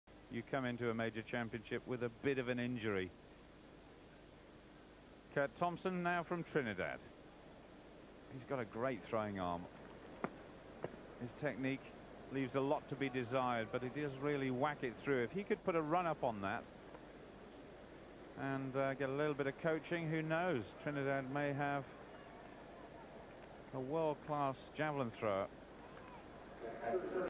6462 Olympic 1996 Javelin Men Kirt Thompson
1996 Olympic Atlanta Javelin Men Qualifying Kirt Thompson 64m12
Buying something on Amazon
1 1  Konstadinos Gatsioudis (GRE) 87.12 — — 87.12 m
2 3  Sergey Makarov (RUS) 85.88 — — 85.88 m
3 6  Seppo Räty (FIN) 83.66 — — 83.66 m
4 7  Raymond Hecht (GER) 83.24 — — 83.24 m
5 9  Peter Blank (GER) 82.68 X — 82.68 m
6 10  Kimmo Kinnunen (FIN) 78.82 X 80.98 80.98 m
7 11  Mick Hill (GBR) 77.12 X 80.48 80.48 m
8 13  Pål Arne Fagernes (NOR) 78.38 X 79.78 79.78 m
9 15  Dave Stephens (USA) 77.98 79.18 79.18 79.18 m
10 18  Emeterio González (CUB) X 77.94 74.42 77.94 m
11 20  Edgar Baumann (PAR) X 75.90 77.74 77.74 m
12 23  Gavin Lovegrove (NZL) X 77.12 X 77.12 m
13 24  Sergey Voynov (UZB) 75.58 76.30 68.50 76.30 m
14 26  Dag Wennlund (SWE) 75.24 X X 75.24 m
15 29  Terry McHugh (IRL) 69.72 X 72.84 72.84 m
16 31  Chu Ki-Young (KOR) X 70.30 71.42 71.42 m
17 33  Kirt Thompson (TRI) 68.02 X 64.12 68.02 m